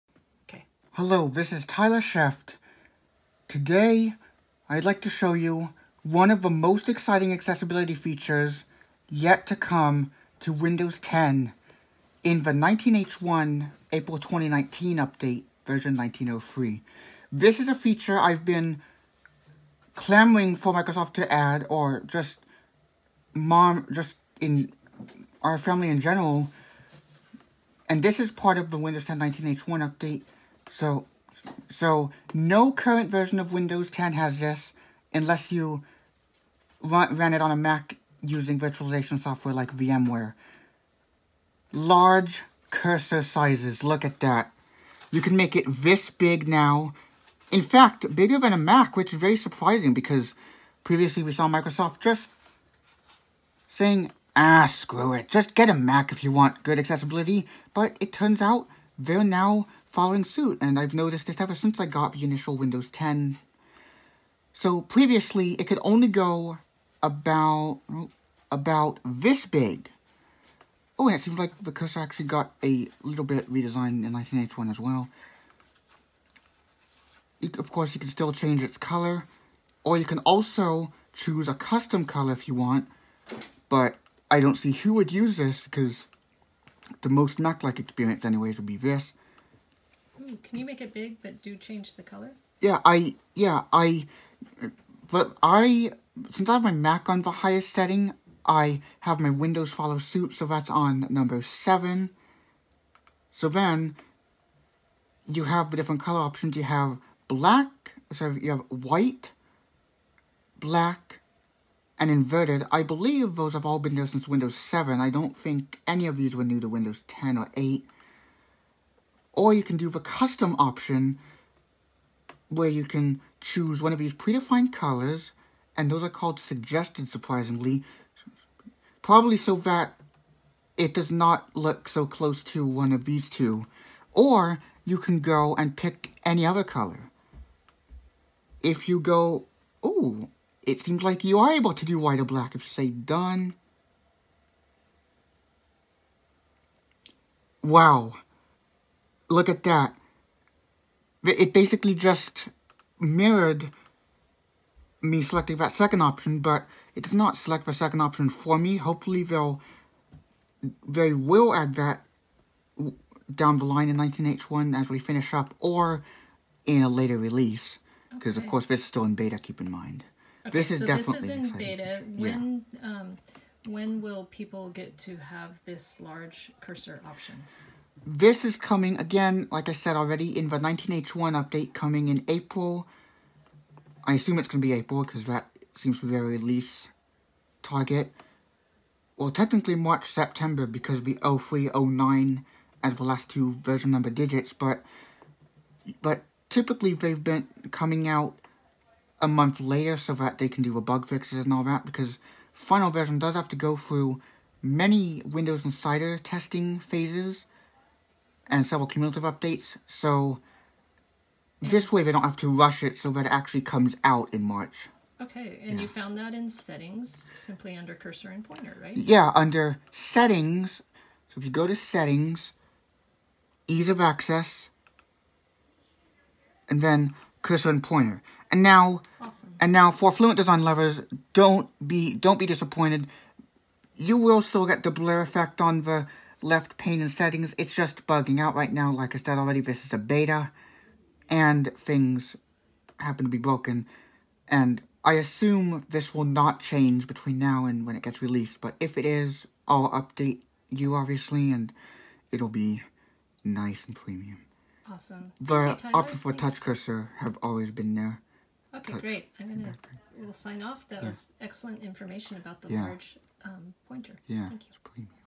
Large Mouse Cursor coming to Windows 10 April 2019 Update
This is a video made by a student about the new large cursor sizes coming in Windows 10 April 2019 Update. Tyler demonstrates how you can make the cursor bigger and change visual options.